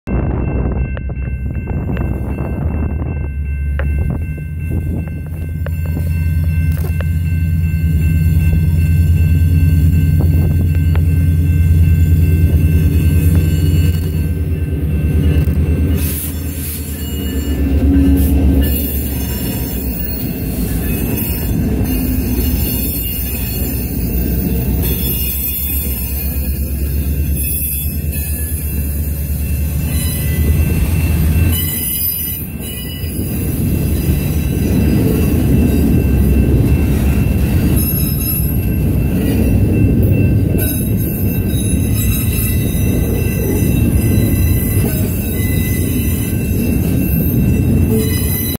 [HD] CPR T97 - CPR 2269 at Parkway (March 12 2017)
CPR 2269 & CPR 2261 (CPR T97) rumbles by Parkway on their way to the interchange